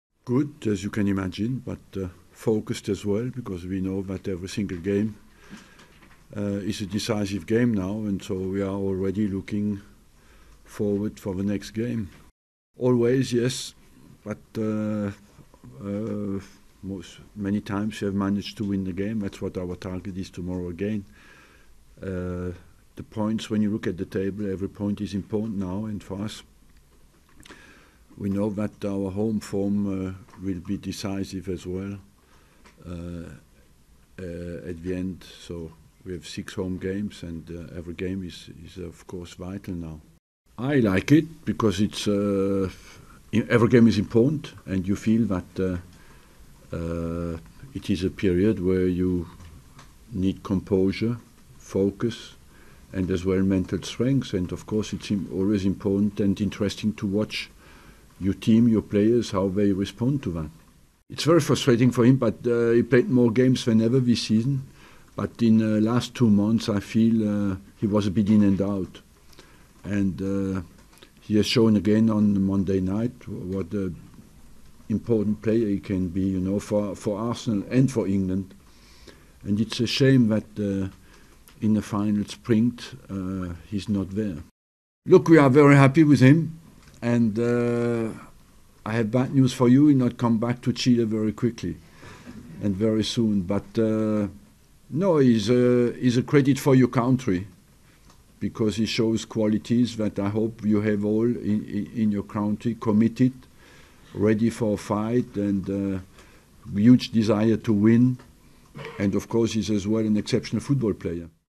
Wenger confirms Oxlade-Chamberlain hamstring injury [AMBIENT] Arsene We
VIDEO: Arsene Wenger looks ahead to West Ham game and their run in until the end of the season -...

                            Wenger confirms Oxlade-Chamberlain hamstring injury [AMBIENT]
                            Arsene Wenger looks ahead to West Ham game and their run in until